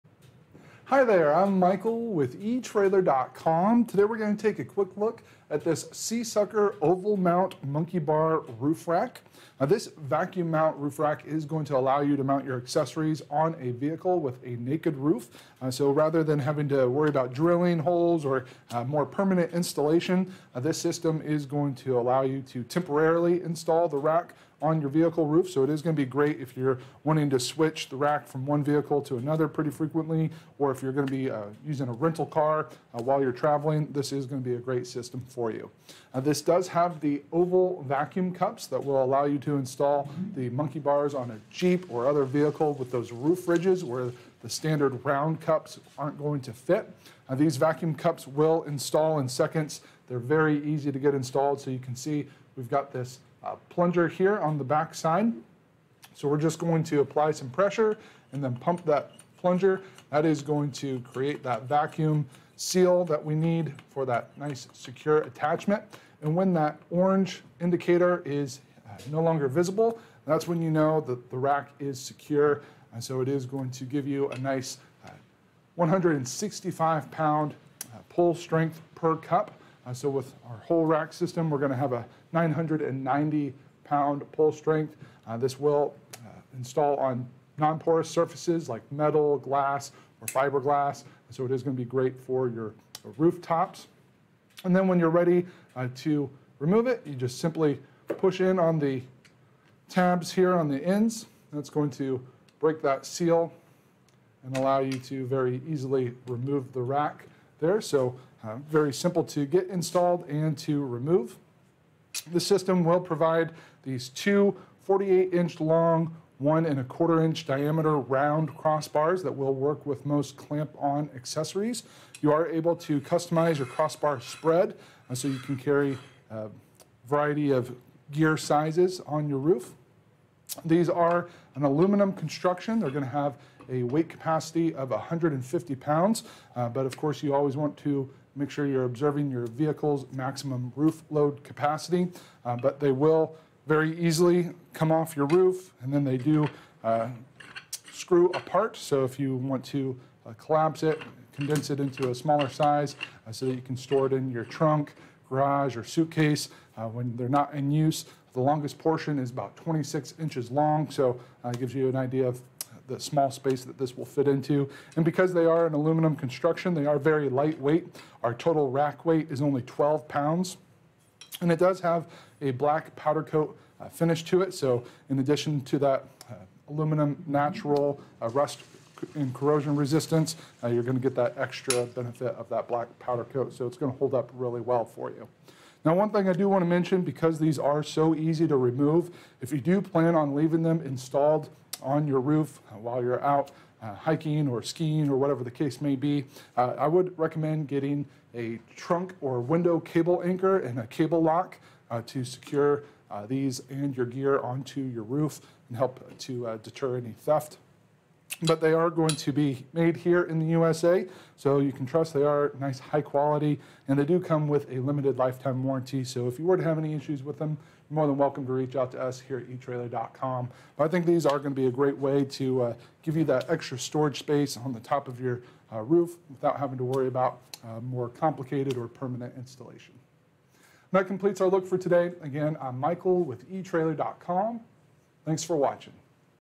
etrailer | All You Need to Know About the SeaSucker Ridge Ready Monkey Bars Roof Rack
Hi there, I'm Michael with etrailer.com.  Today we're gonna take a quick look at this SeaSucker Oval Mount Monkey bar roof rack.  Now this vacuum mount roof rack is going to allow you to mount your accessories on a vehicle with a naked roof.  So rather than having to worry about drilling holes or more permanent installation this system is going to allow you to temporarily install the rack on your vehicle roof.  So it is gonna be great if you're wanting to switch the rack from one vehicle to another pretty frequently.  Or if you're gonna be using a rental car while you're traveling this is gonna be a great system for you.

This does have the oval vacuum cups that will allow you to install the monkey bars on a Jeep or other vehicle with those roof ridges where the standard round cups aren't going to fit.  These vacuum cups will install in seconds.  They're very easy to get installed.  So you can see we've got this plunger here on the back side.  So we're just going to apply some pressure and then pump that plunger that is going to create that vacuum seal that we need for that nice secure attachment.

And when that orange indicator is no longer visible that's when you know that the rack is secure.  And so it is going to give you a nice 165 pound pull strength per cup.  And so with our whole rack system we're gonna have a 990 pound pull strength.  This will install on non-porous surfaces like metal, glass or fiberglass.  And so it is gonna be great for your rooftops.

And then when you're ready to remove it you just simply push in on the tabs here on the ends.  That's going to break that seal and allow you to very easily remove the rack there.  So very simple to get installed and to remove.  The system will provide these two 48 inch long one and a quarter inch diameter round crossbars that will work with most Clamp On accessories, you are able to customize your cross bar spread so you can carry a variety of gear sizes on your roof.  These are an aluminum construction.

They're gonna have a weight capacity of 150 pounds.  But of course, you always want to make sure you're observing your vehicle's maximum roof load capacity.  But they will very easily come off your roof and then they do screw apart.  So if you want to collapse it, condense it into a smaller size so that you can store it in your trunk, garage, or suitcase when they're not in use.  The longest portion is about 26 inches long.  So it gives you an idea of the small space that this will fit into.  And because they are an aluminum construction they are very lightweight.  Our total rack weight is only 12 pounds and it does have a black powder coat finish to it.  So in addition to that aluminum natural rust and corrosion resistance now you're gonna get that extra benefit of that black powder coat.  So it's gonna hold up really well for you.  Now one thing I do wanna mention because these are so easy to remove, if you do plan on leaving them installed on your roof while you're out hiking or skiing or whatever the case may be I would recommend getting a trunk or window cable anchor and a cable lock to secure these and your gear onto your roof and help to deter any theft.  But they are going to be made here in the USA so you can trust they are nice, high quality and they do come with a limited lifetime warranty.  So if you we're to have any issues with them you're more than welcome to reach out to us here at etrailer.com.  But I think these are gonna be a great way to give you that extra storage space on the top of your roof without having to worry about more complicated or permanent installation.  That completes our look for today.